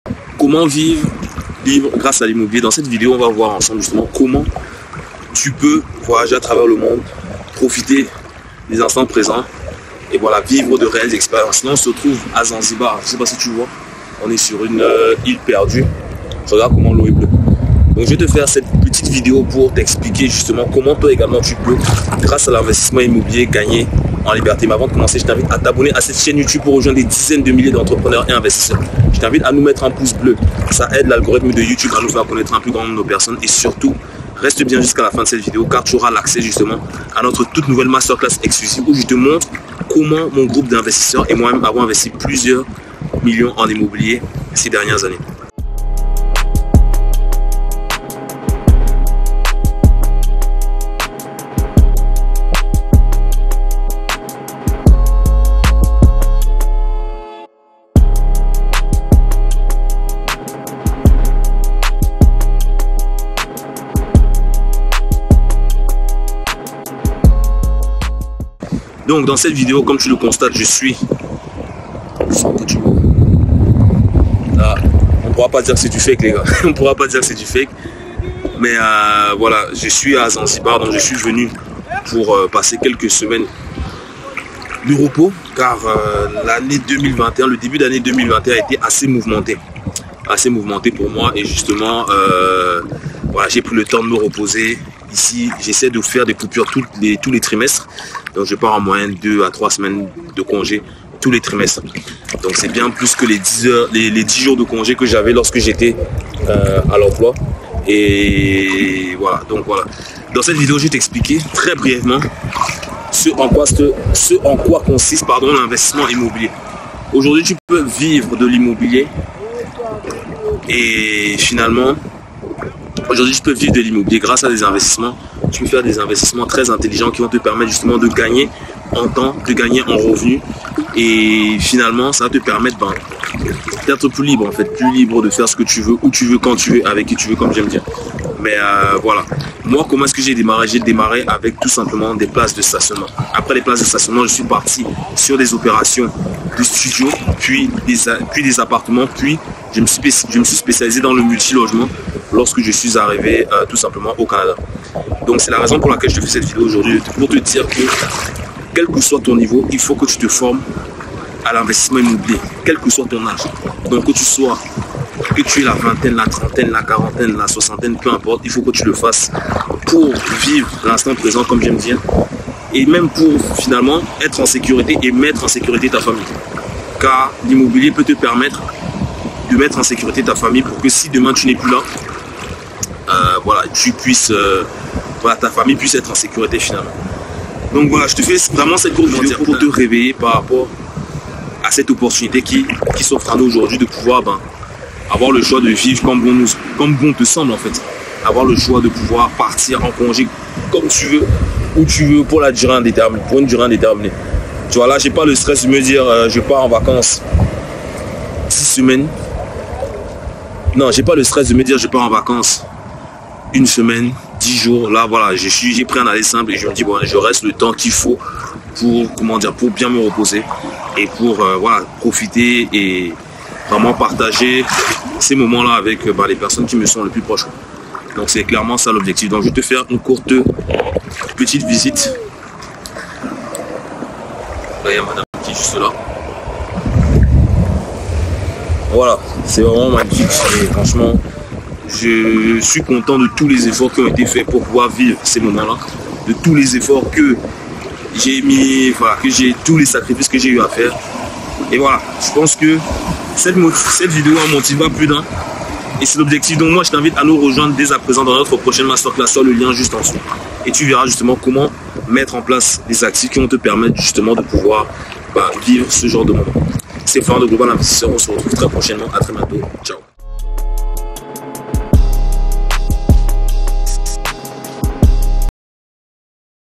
Va Chercher Ta Liberté Car Toi Aussi Tu L'as Mérite !
Dans cette nouvelle vidéo je te parle de comment vivre libre grâce à l'investissement immobilier

Avertissement